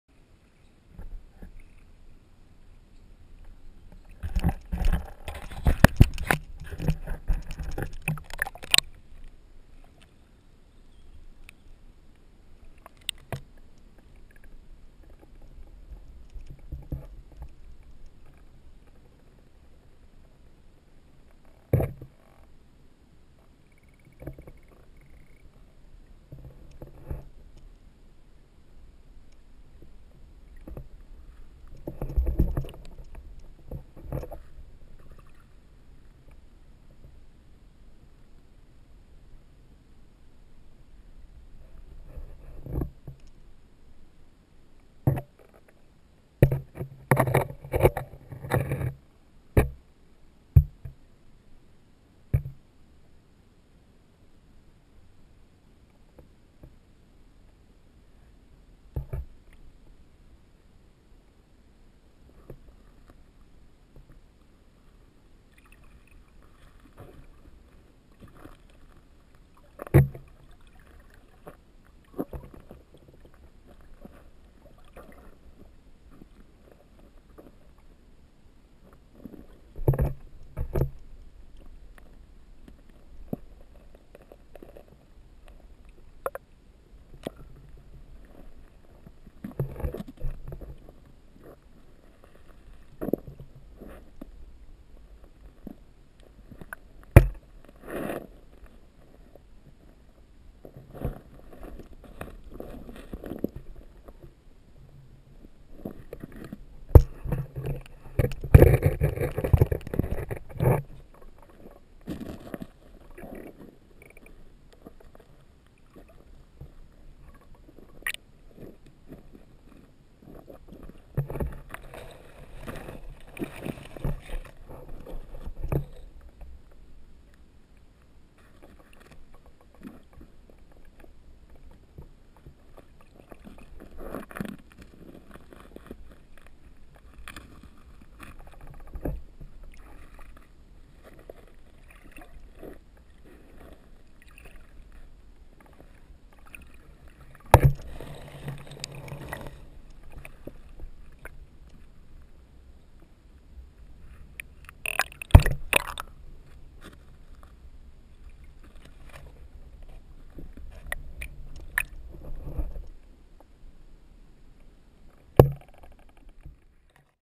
rybka
Brave little fish I came across, on a fishing trip with my neighbor Janek.